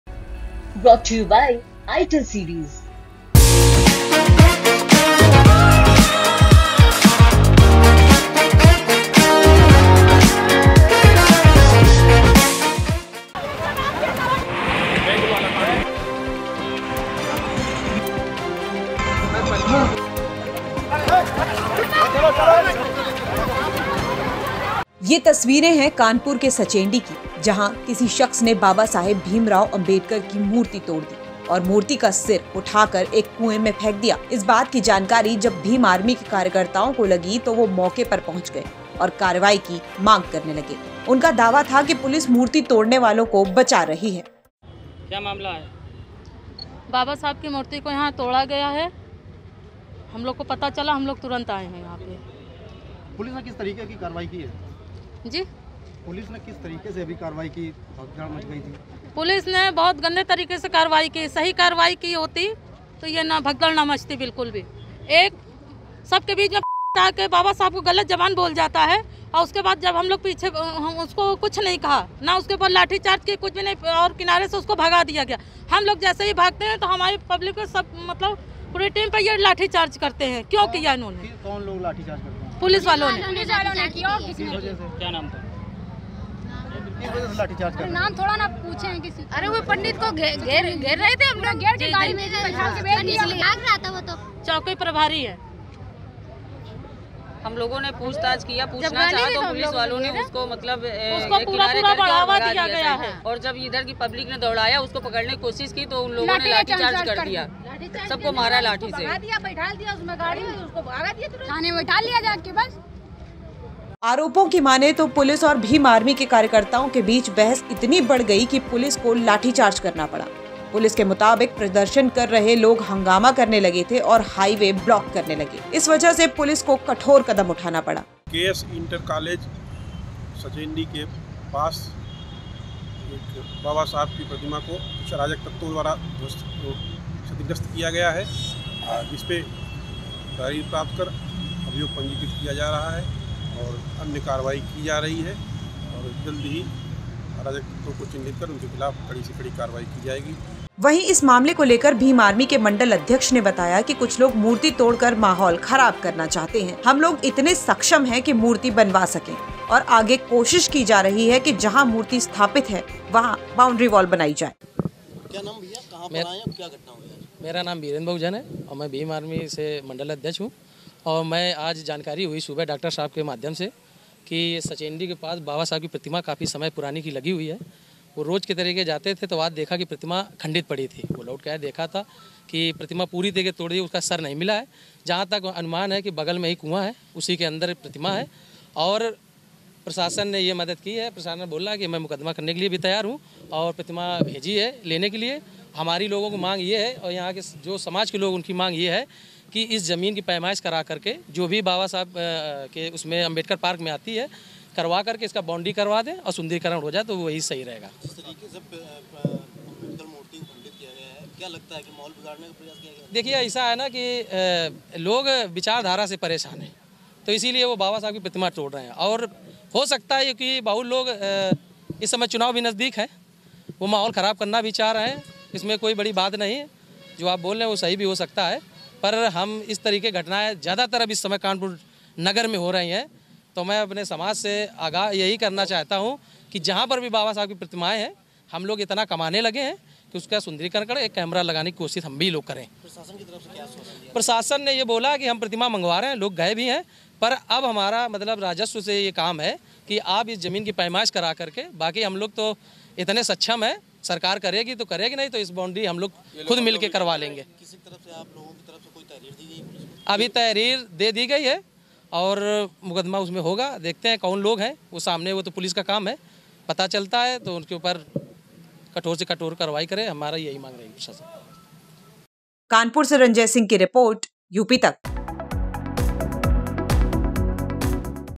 Bhim Army Kanpur: भीम आर्मी वालों को कानपुर पुलिस ने क्यों दौड़ा-दौड़ा कर पीटा?
ये तस्वीरें हैं कानपुर के संचेड़ी की, जहाँ किसी शख्स ने बाबा साहब भीम राव अंबेडकर की मूर्ति तोड़ दी और मूर्ति का सिर उठा कर एक कुंवे में फेंक दिया.. इस बात की जानकारी जब भीम आर्मी के कार्यकर्ताओं को लगी तो वो मौके पर पहुँच गए और कार्रवाई की मांग करने लगे.. उनका दावा था कि पुलिस मूर्ति तोड़ने वालों को बचा रही है।